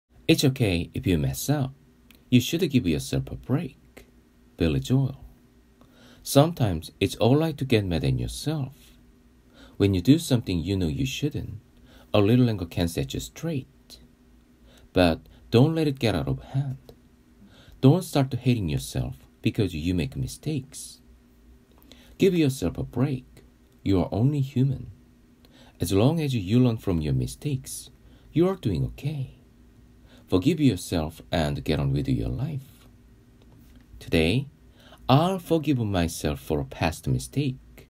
20190104
“It’s okay if you mess up.
You should give yourself a break.”
Billy Joel

Sometimes it’s all right to get mad at yourself.
When you do something you know you shouldn’t, a little anger can set you straight.
But don’t let it get out of hand.
Don’t start hating yourself because you make mistakes.
Give yourself a break; you’re only human.
As long as you learn from your mistakes, you’re doing okay.
Forgive yourself and get on with your life.

Today
I’ll forgive myself for a past mistake.